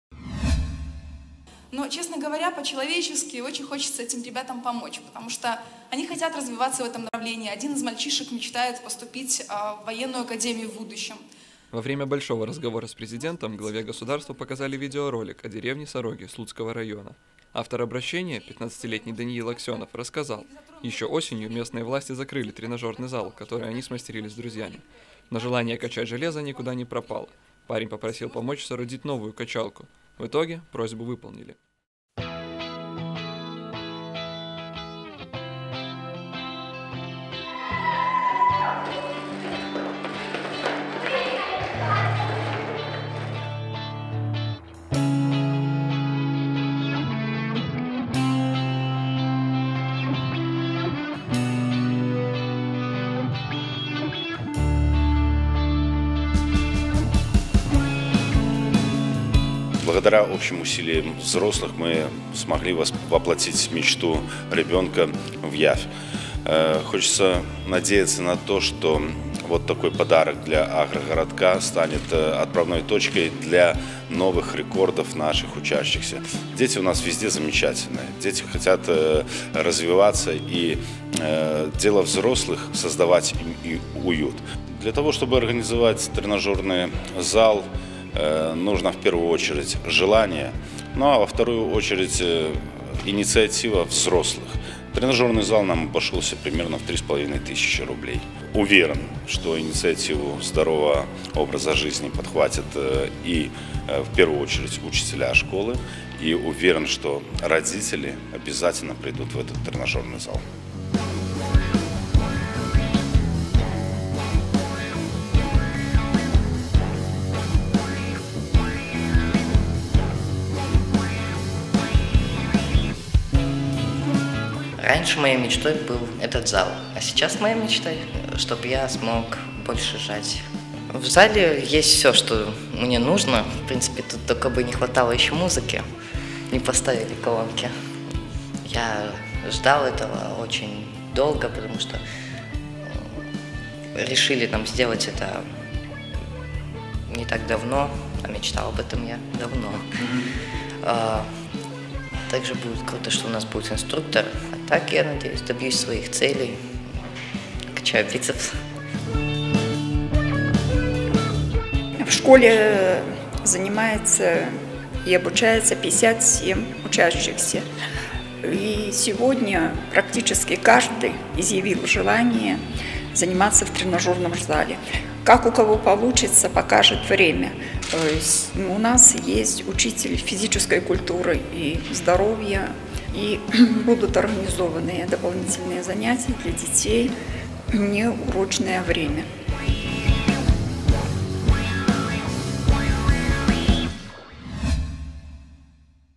Новый тренажерный зал открылся в деревне Сороги Слуцкого района
Во время «Большого разговора с Президентом» Главе государства показали видеоролик о деревне Сороги Слуцкого района. Автор обращения, 15-летний Даниил Аксенов, рассказал: еще осенью местные власти закрыли тренажерный зал, который они смастерили с друзьями. Парень попросил помощи. О небольшом агрогородке под Слуцком узнала вся страна. Уже через две недели мы оказались в Сорогах на открытии долгожданного тренажерного зала.